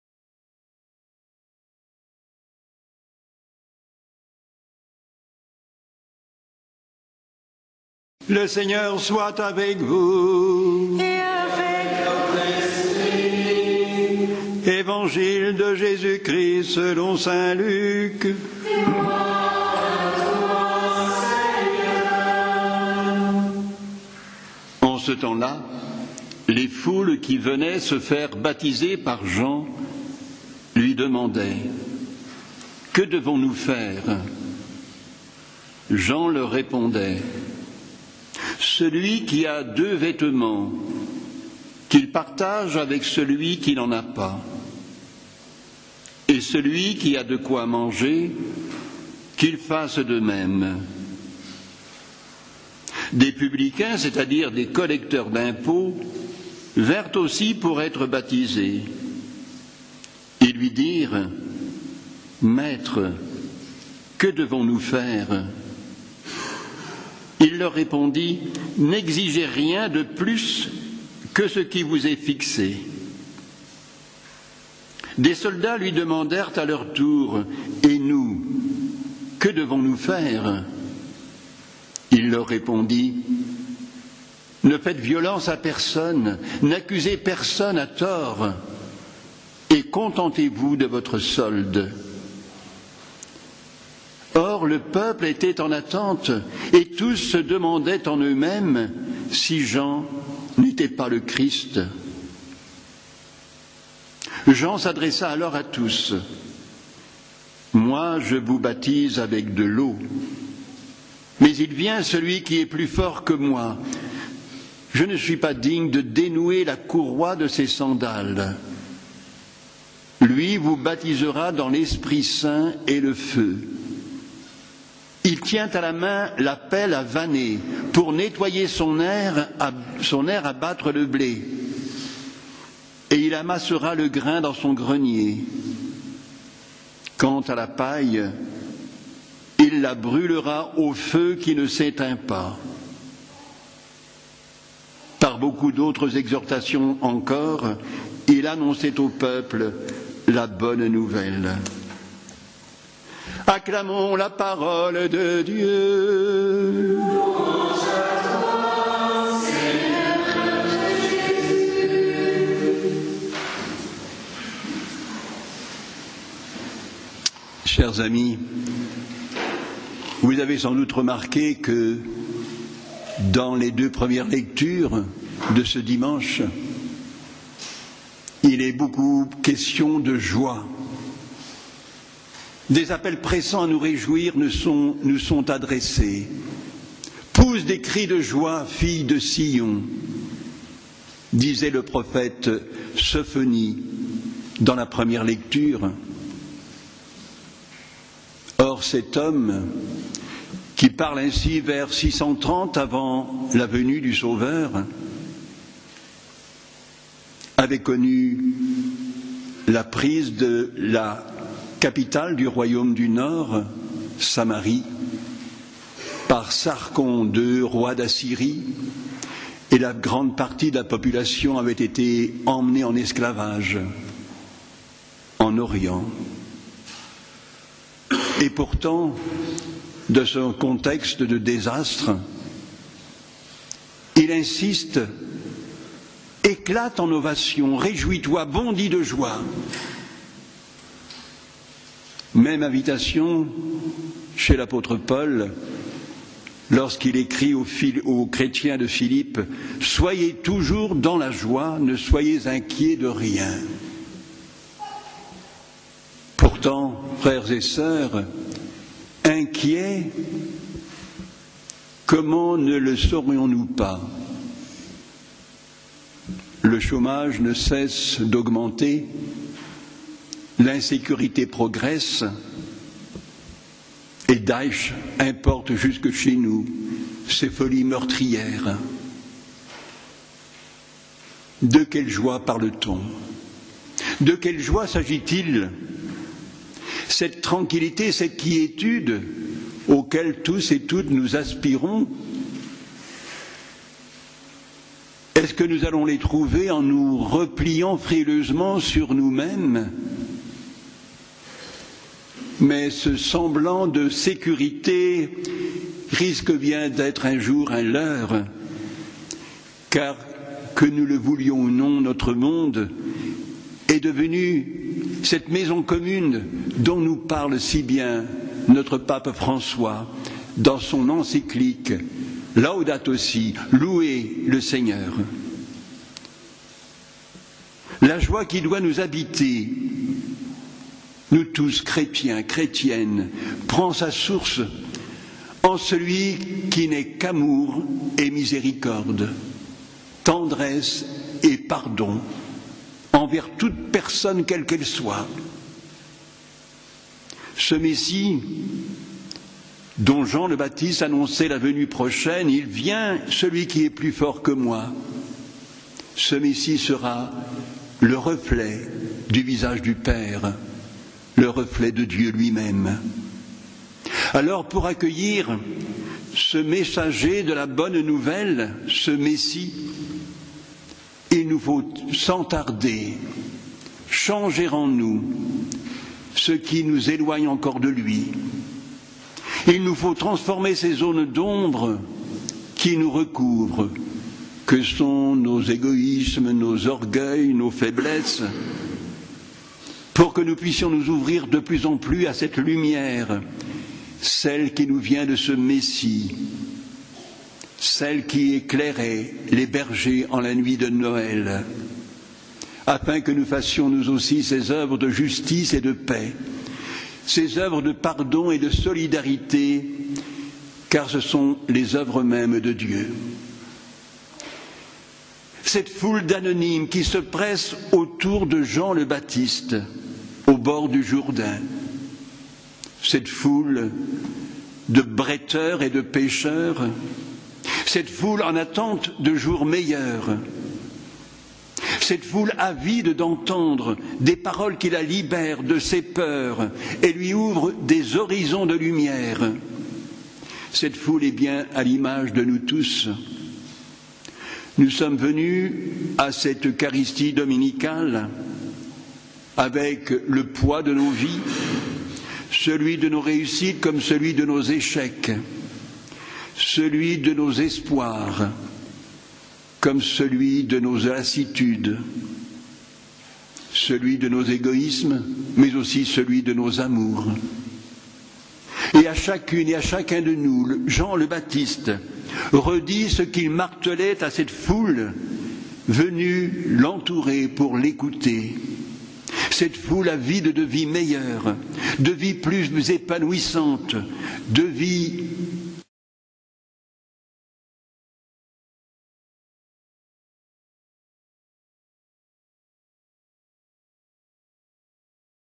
Soyons dans la joie, le Christ est parmi nous - Père François Vanier - Relais Pascal
Soyons dans la joie, le Christ est parmi nous
Père François Vanier C.Ss.R - 3ème dimanche de l'Avent 2015 - Temps de l'Avent - Année liturgique C
Relais Pascal - Communauté des Béatitudes - Les Sables d'Olonne
Lectures : So 3, 14-18a ;  Is 12, 2-6 ; Ph 4, 4-7 ; Lc 3, 10-18